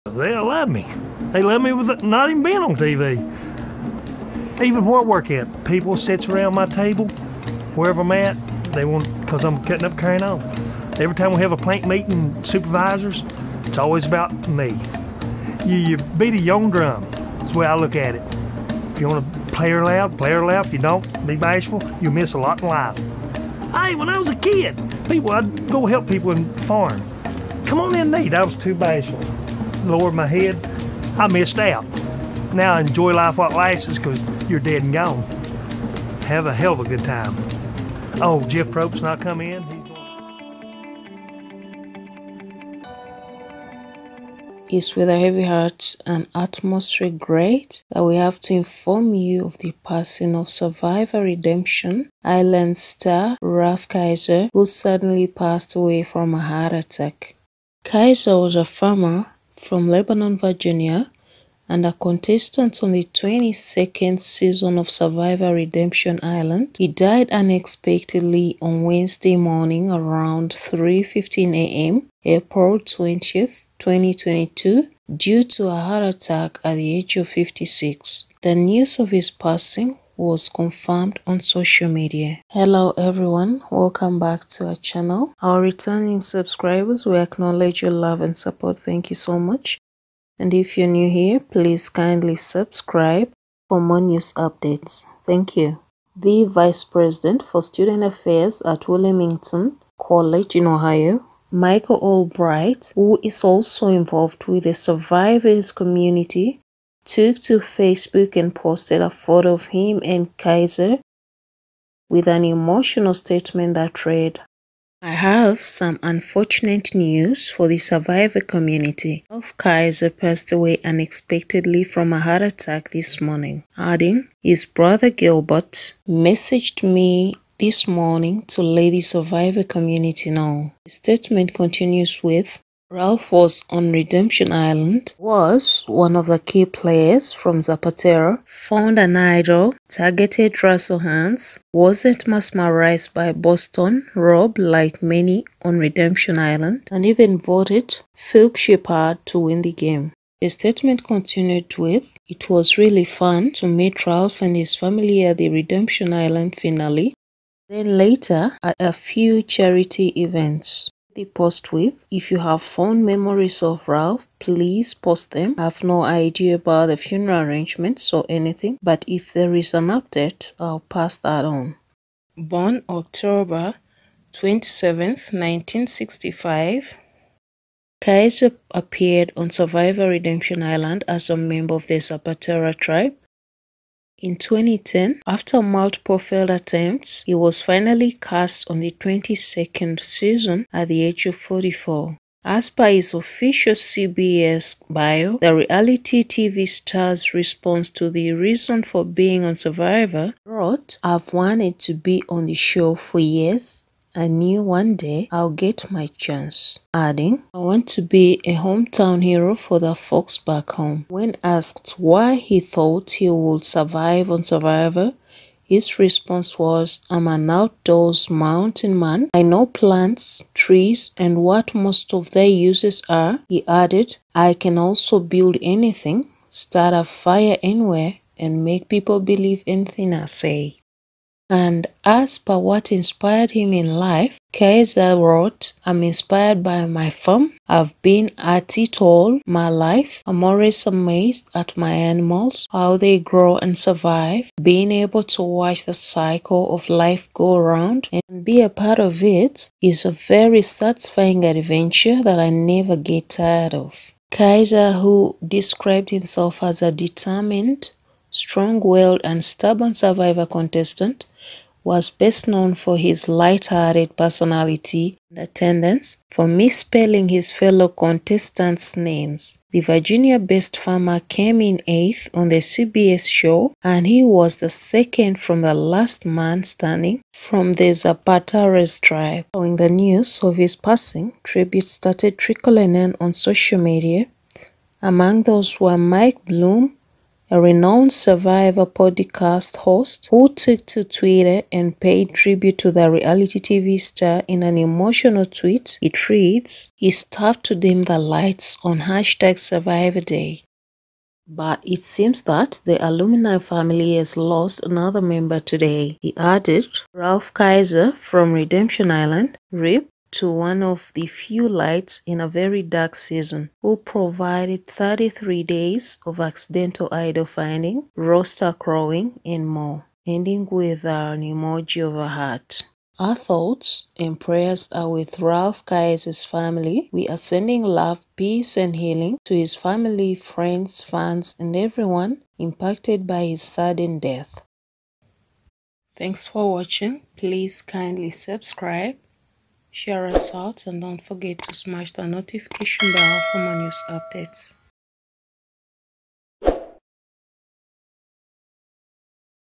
Sad-Survivor Redemption Island's star Ralph Kiser suddenly Passed away from a heart attack At 56
It's with a heavy heart and utmost regret that we have to inform you of the passing of  Survivor Redemption Island's star Ralph Kiser who suddenly Passed away from a heart attack.

Kiser was a farmer from Lebanon, Virginia & a contestant on the 22nd season of Survivor Redemption Island died unexpectedly on Wednesday morning around 3:15 am  April 20, 2022, due to a heart attack at the age of 56. The news of his passing was confirmed on social media.

The Vice President for Student Affairs at Wilmington College in Ohio. Michael Allbright who is also involved with the “Survivor” community took to Facebook and posted a photo of him & Kiser with an emotional statement that read

''I have some unfortunate news for the Survivor community.  Ralph Kiser passed away unexpectedly from a heart attack this morning.  His brother Gilbert messaged me this morning to let the Survivor community know.''

''Ralph was on Redemption Island, was one of the key players from Zapatera, found an idol, targeted Russell Hantz, wasn't mesmerized by Boston Rob like many on Redemption Island, and even voted for Phillip Sheppard to win the game.''

''It was really fun to meet Ralph and his family at the Redemption Island finale, and then later at a few charity events.''

''If you have fond memories of Ralph please post them.  I have no idea about funeral arrangements or anything, but if there is an update I will pass that on.''

Born October 27, 1965, Kiser appeared on Survivor Redemption Island as a member of the Zapatera tribe in 2010 after multiple failed attempts he was finally cast on the 22nd season at the age of 44.

As per his official  CBS Bio, the reality star's response to the reason for being on SURVIVOR:
He wrote '' I've wanted to be on the show for years and knew one day I'd get my chance. I want to be a hometown hero for the folks back home!''

When asked why he thought he will "Survive" on survivor his response was  ''I'm an outdoors mountain man. I know plants, trees, and what most of their uses are.'' adding  ''I can also build anything; start a fire anywhere and make people believe anything I say''

 And as per what inspired him in life  Kiser wrote

"I'm inspired by my farm; I've been at it all my life. I'm always amazed at my animals - how they grow and survive. Being able to watch the circle of life go-'round and be a part of it is a very satisfying adventure that I never get tired of.''

Kiser who ''described himself as a determined, strong-willed, and stubborn SURVIVOR Contestant'' was best known for his light-hearted personality and a tendency for misspelling his fellow contestants' names. - The Virginia-based farmer came in eighth on the CBS show and he was the second from the last man standing from the Zapateras.

Following the news of his passing tributes started trickling on social media among those was Mike Bloom a renowned Suvirvor podcast host who took to Twitter and paid tribute to the reality Tv star in an emotional tweet it reads

''It's tough to dim the lights on Survivor day, but it seems that the alumni family has lost another member today''

''Ralph Kiser from Redemption Island. R.I.P. to one of the few lights in a very dark season, who provided 33 days of accidental idol finding, rooster crowing & more❤️''

Our thoughts and prayers are with Ralph Kiser's family, We are sending love peace, and healing to his family, friends, fans, and everyone impacted by his sudden death.''